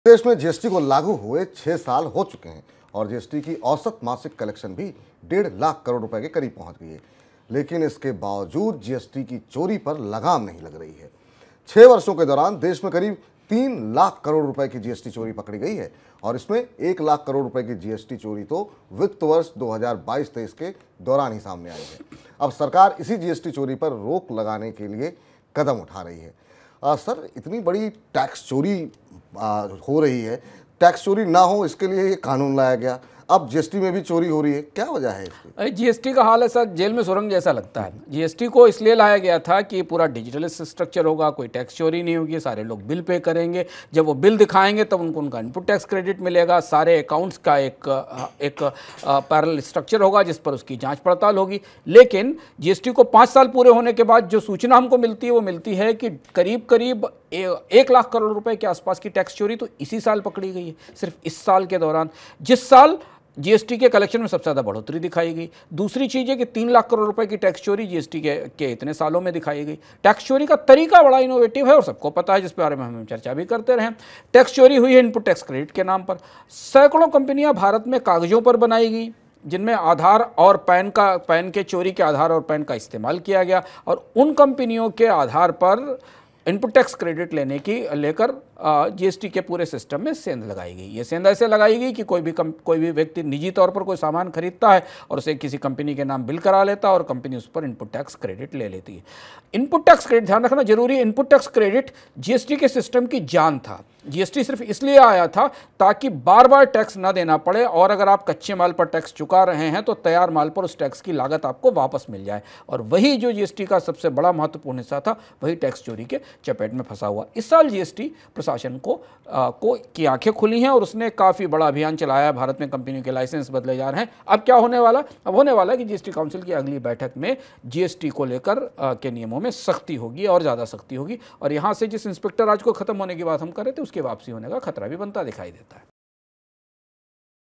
कैसे हो गई 3 लाख करोड़ की GST चोरी? Explained | Anshuman Tiwari | Money9
देश में GST को लागू हुए 6 साल हो चुके हैं और जीएसटी की औसत मासिक कलेक्शन भी 1.5 लाख करोड़ रुपए के करीब पहुंच गई है. लेकिन इसके बावजूद जीएसटी की चोरी पर लगाम नहीं लग रही. 6 वर्षों के दौरान देश में करीब 3 लाख करोड़ रुपए की जीएसटी चोरी पकड़ी गई है और इसमें 1 लाख करोड़ रुपए की जीएसटी चोरी तो वित्तवर्ष 2022-23 के दौरान ही सामने आई है. अब सरकार इसी जीएसटी चोरी पर रोक लगाने के लिए कदम उठा रही है.